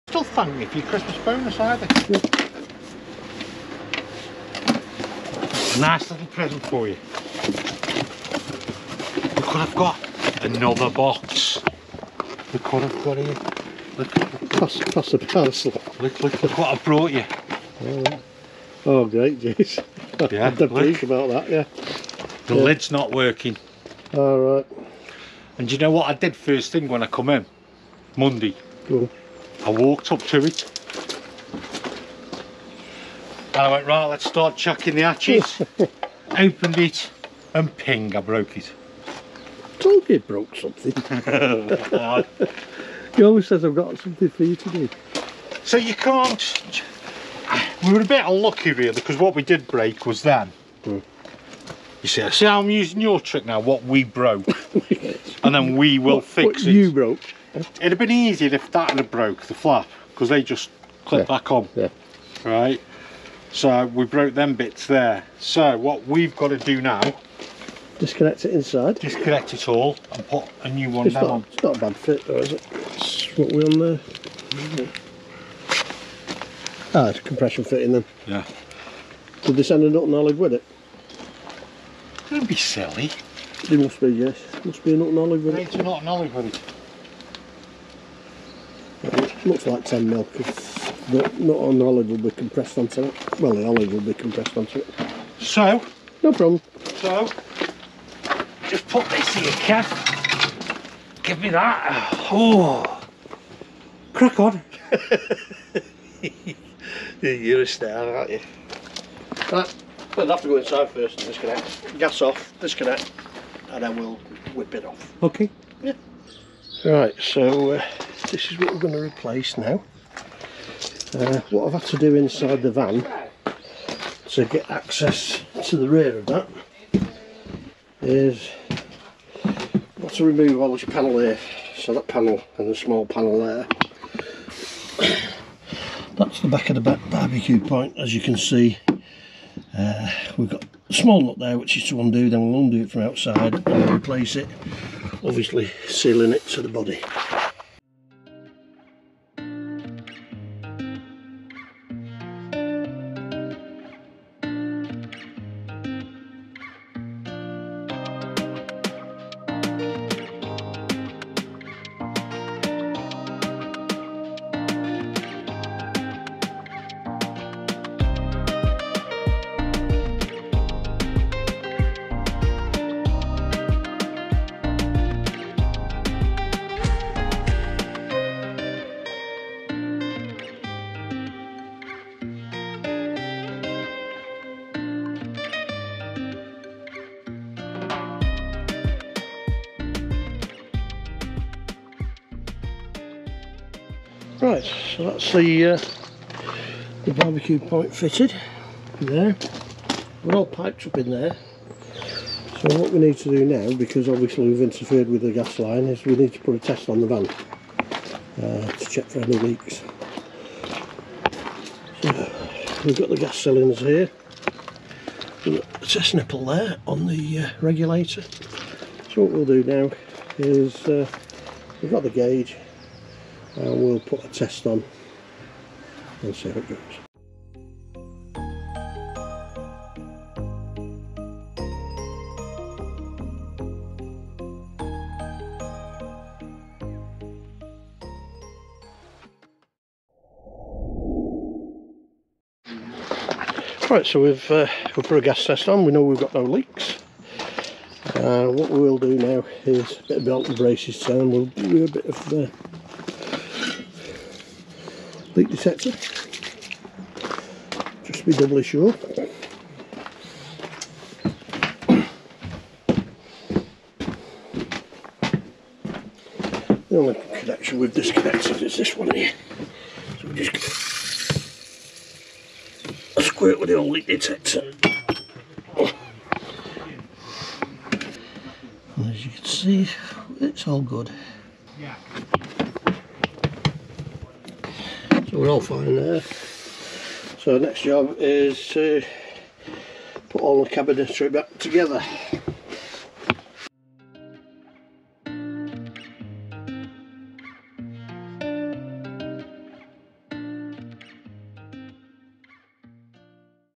I BROKE IT ! Changing Motorhome Gas BBQ Point.
Changing The Motorhome and LPG Gas Outside BBQ Point.
Another week of a used Motorhome and Campervan Dealer and some of the jobs we do.

Looking to Sell Your Motorhome - We Buy all types of Motorhomes and Campervans.

TikTok.      The Motorhome Man
Instagram The Motorhome Man